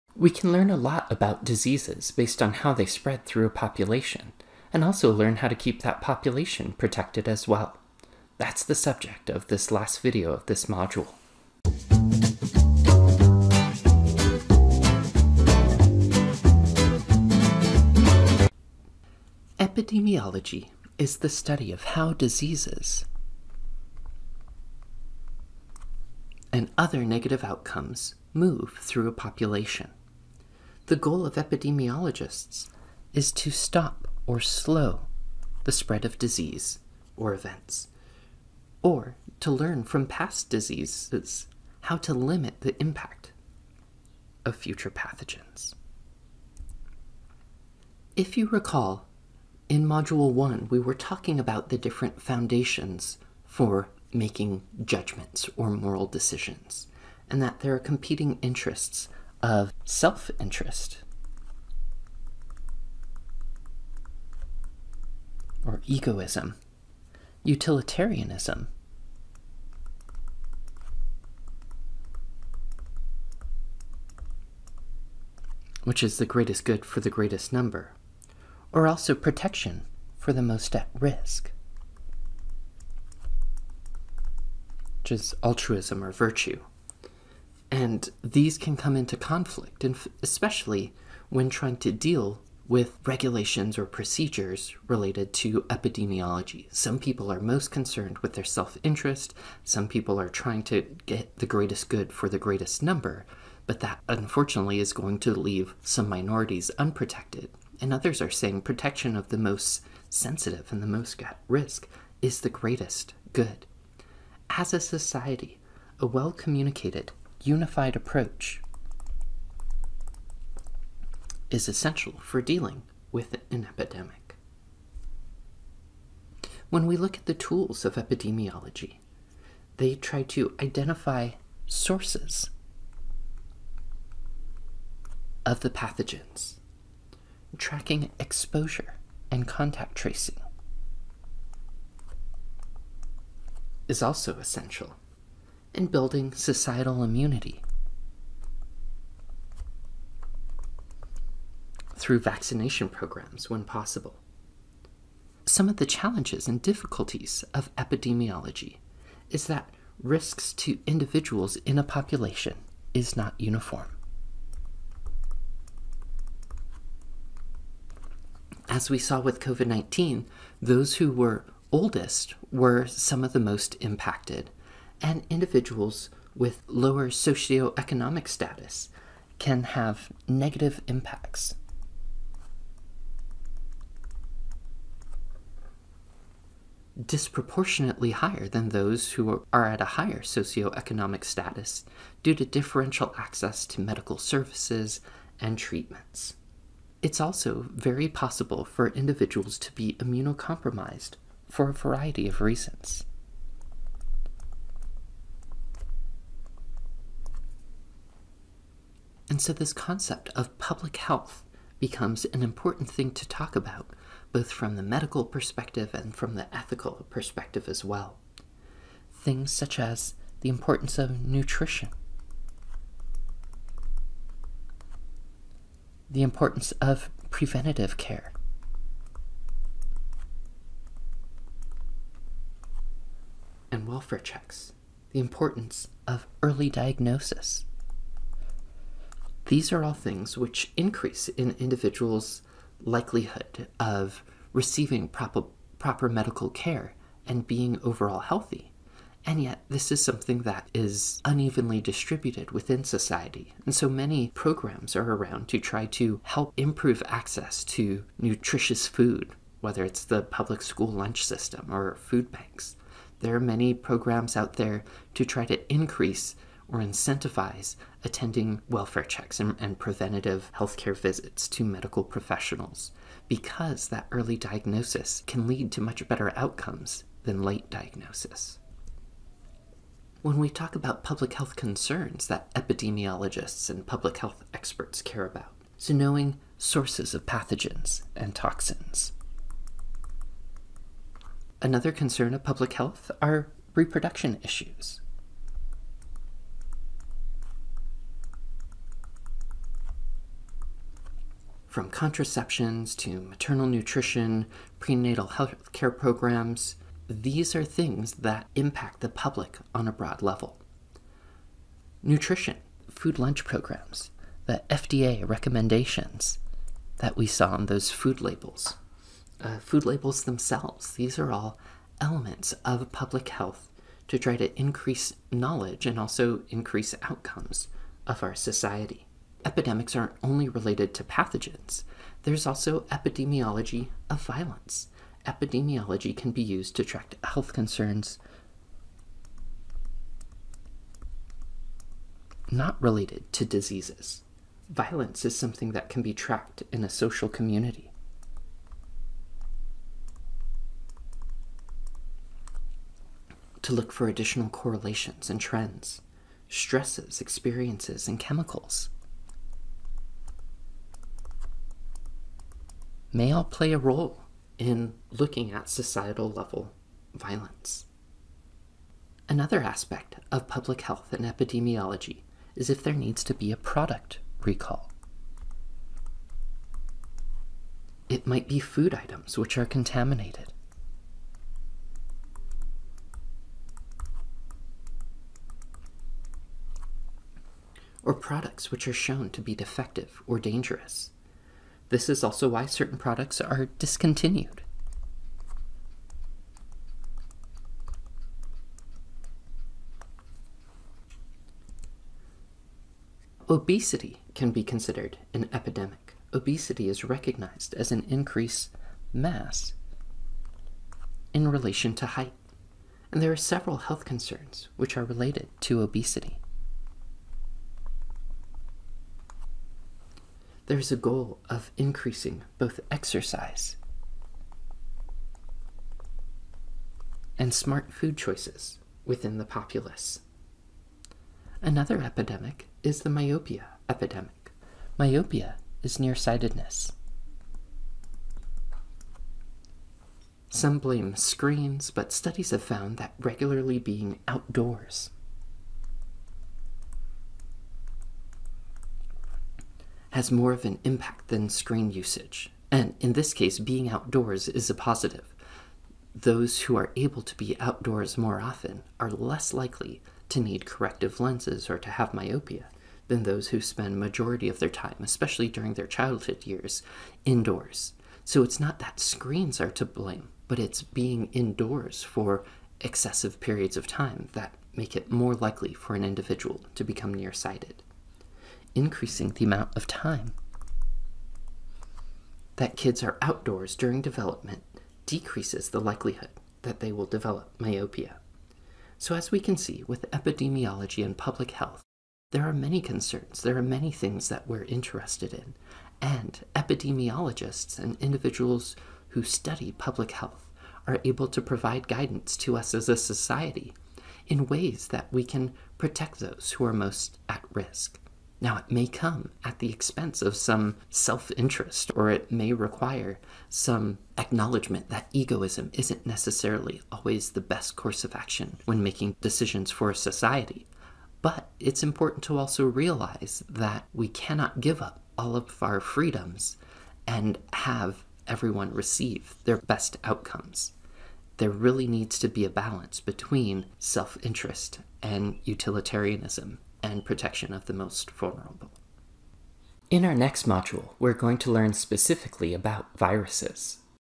Lecture 11.3 – Epidemiology and Public Health – Biol 101
In this video we will be discussing the spread of diseases through a population and the goal of Public Health initiatives. Access to the lecture notes can be found